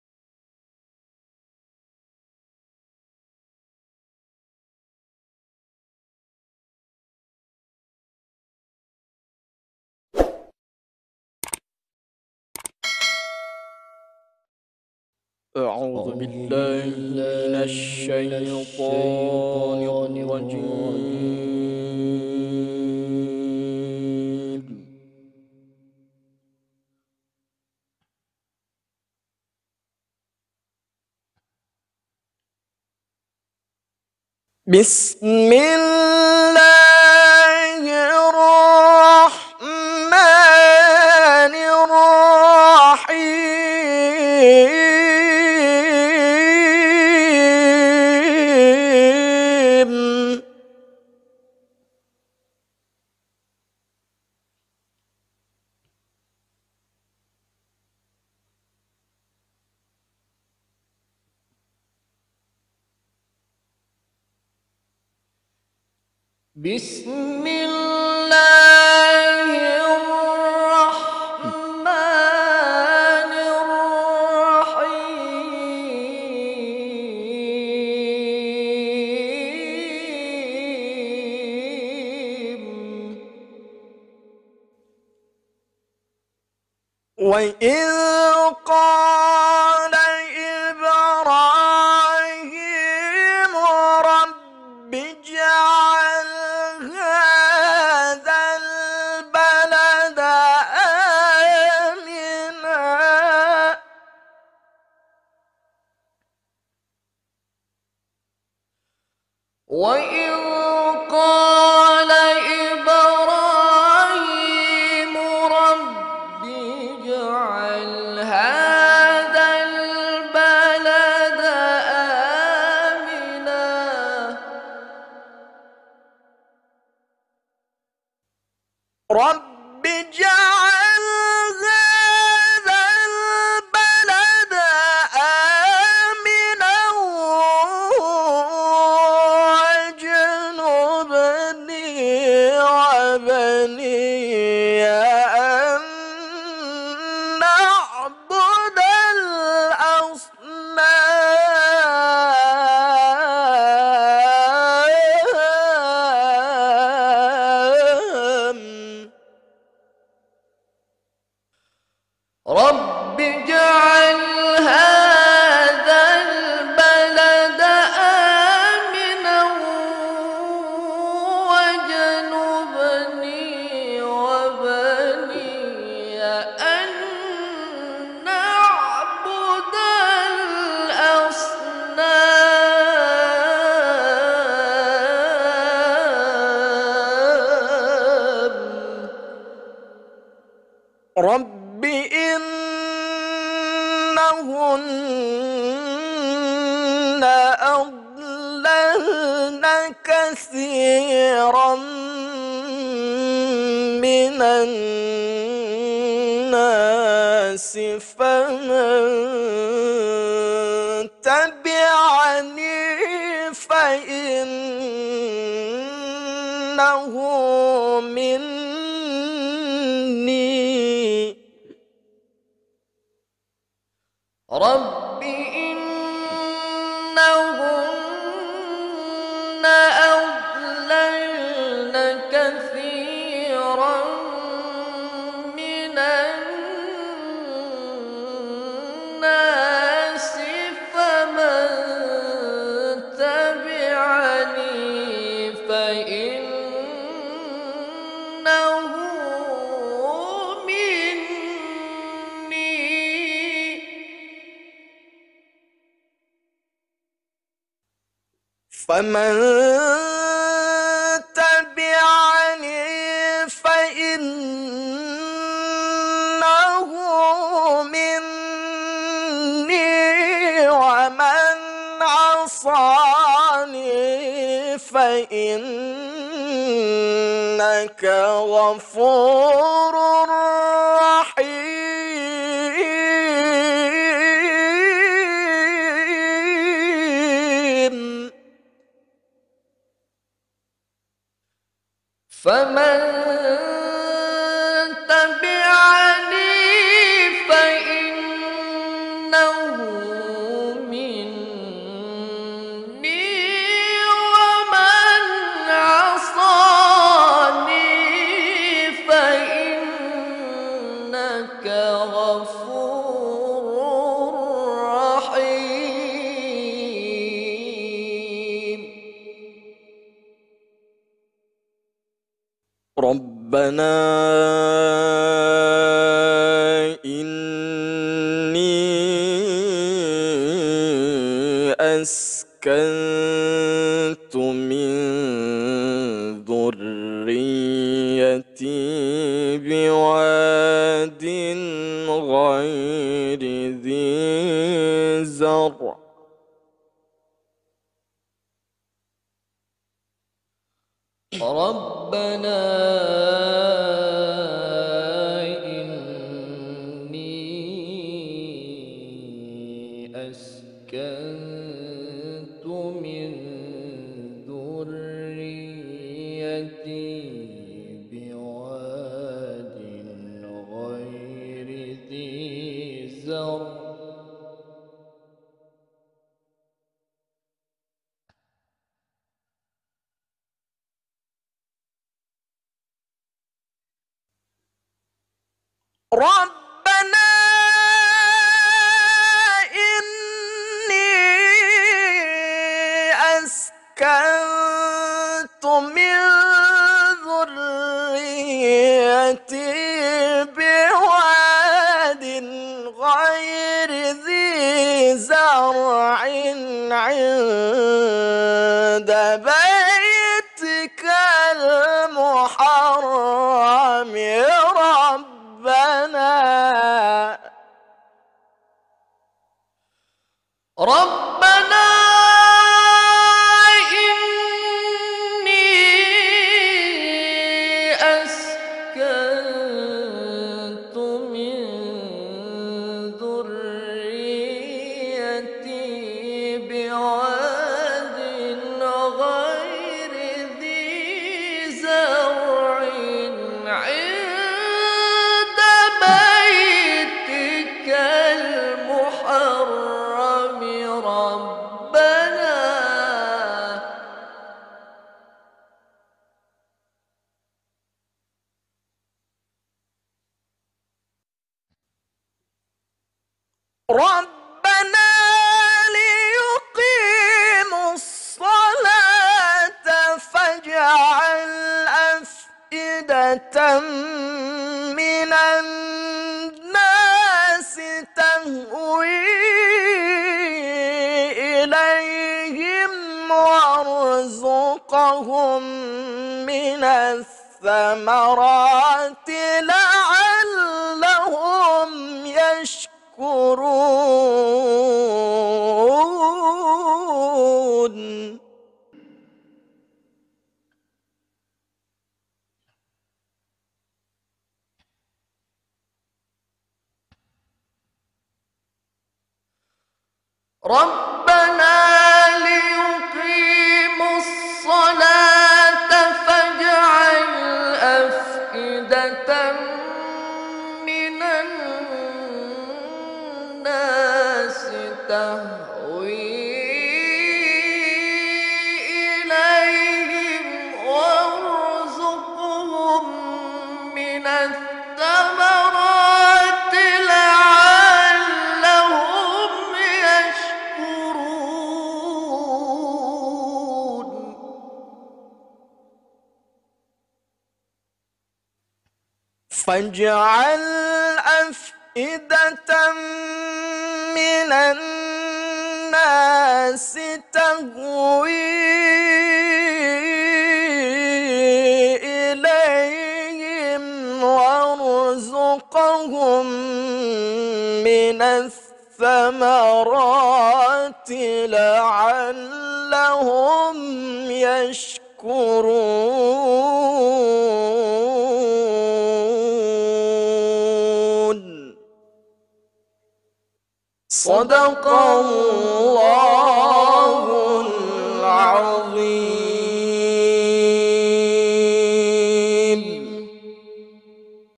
DUET TILAWAH QORI INTERNASIONAL SYAMSURI FIRDAUS DAN AHMAD KHAIRI NOVANDRA
Ustadz Syamsuri Firdaus adalah seorang Qori Internasional yang berasal dari Kab. Bima Provinsi Nusa Tenggara Barat. Adapun prestasi beliau
1. Juara 1 MTQ Internasional di Singapura tahun 2012
2. Juara 1 MTQ Internasional di Turki tahun 2019
Suatu kebanggaan bisa berkolaborasi dengan Ustadz Syamsuri Firdaus. Semoga Ustadz Syamsuri tetap dalam kondisi sehat wal afi'at. Aminnn

Surah Ibrahim ayat 35-37

Terimakasih